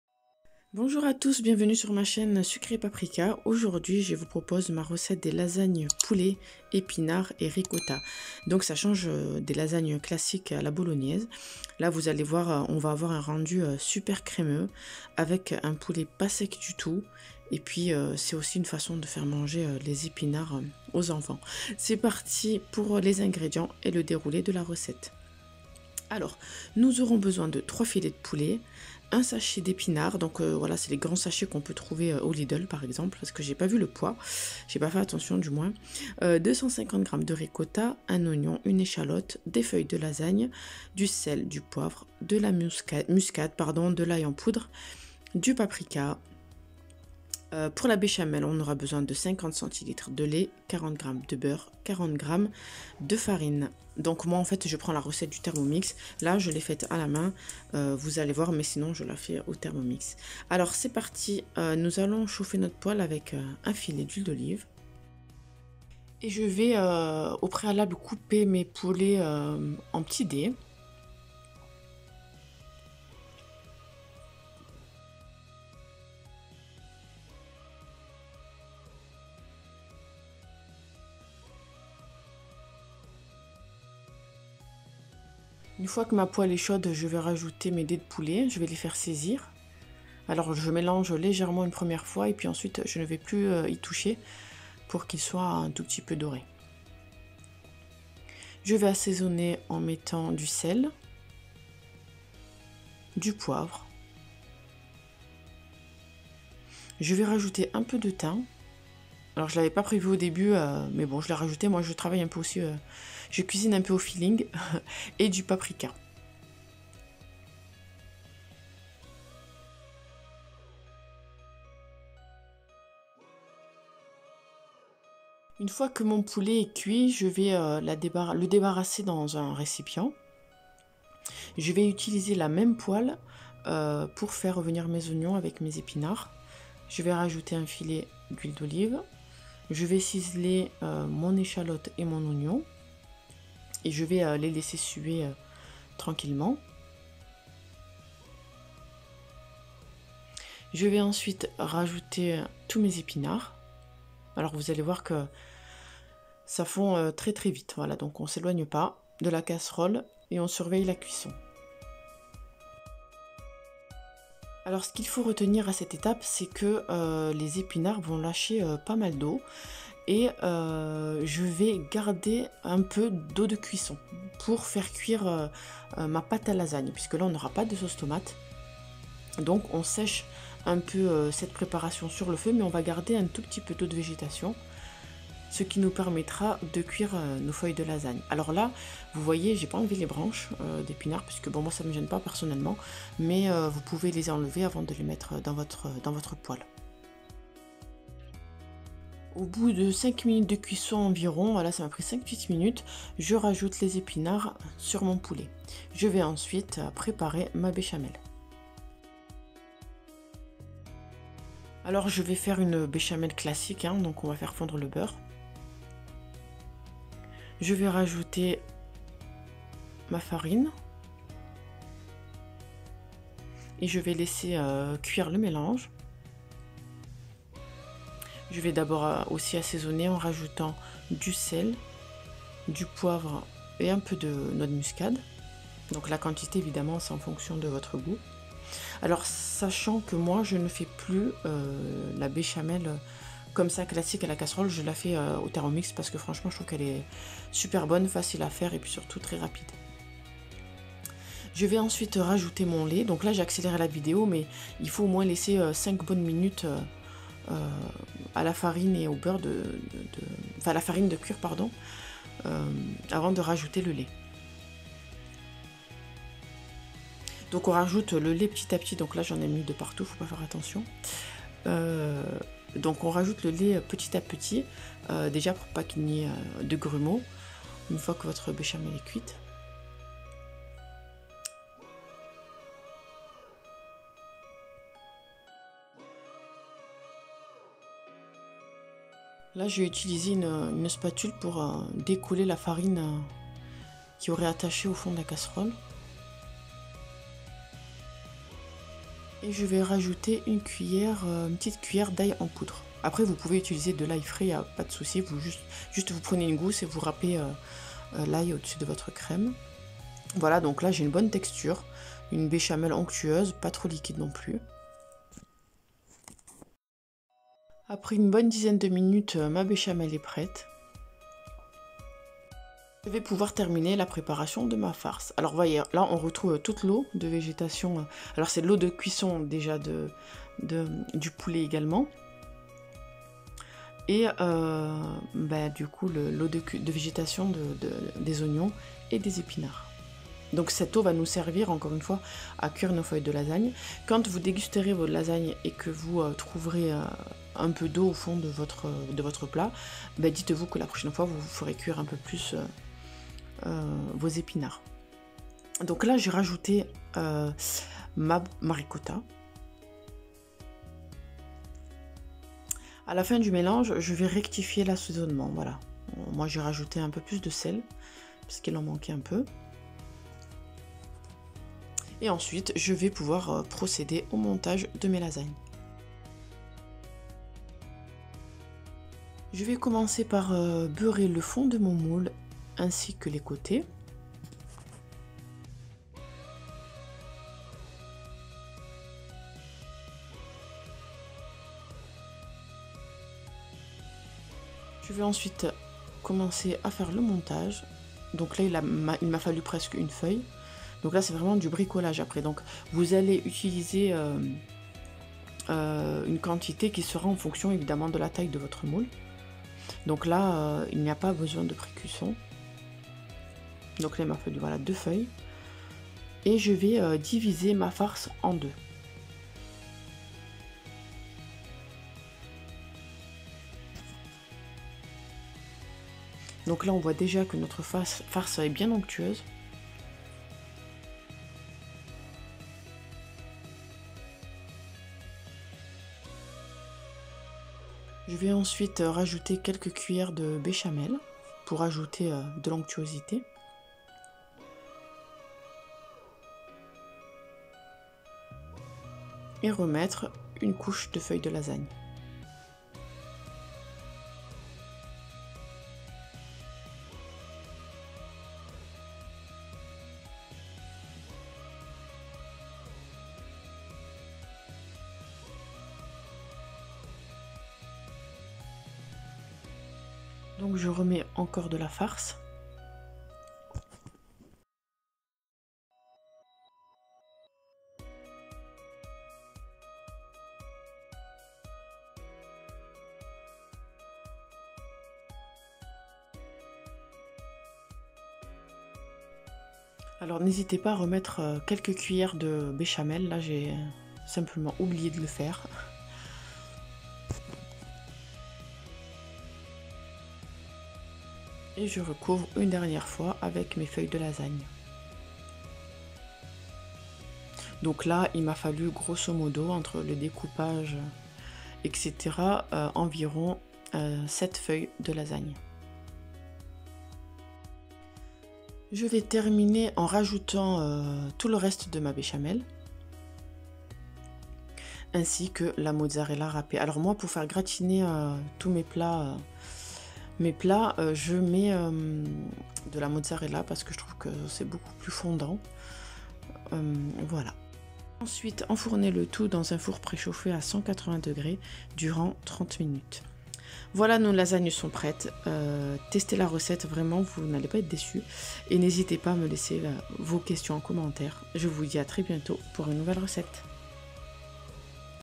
Lasagnes au Poulet, épinards et Ricotta | Très fondantes !!
Bonjour à tous! Aujourd'hui pour changer des Lasagnes classiques je vous propose celleq au Poulet, épinards et Ricotta !

Elles sont superbement fondantes et pas sèches du tout !

👍👍N'oubliez pas pour m'encourager à LIKEZ mes vidéos ! ❤️❤️

⬇️Ingredients Pour 4 personnes :
- 3 filets de poulet
- des feuilles de lasagnes
- 1 sachet d'épinards frais
- 1 oignon
- 1 échalotte
- 50cl de lait
-40g de beurre
-40gr de farine
- 250gr de ricotta
-sel, poivre, muscade, Ail en poudre, thym, paprika.
-Mozzarella râpée

Bon ap, 😉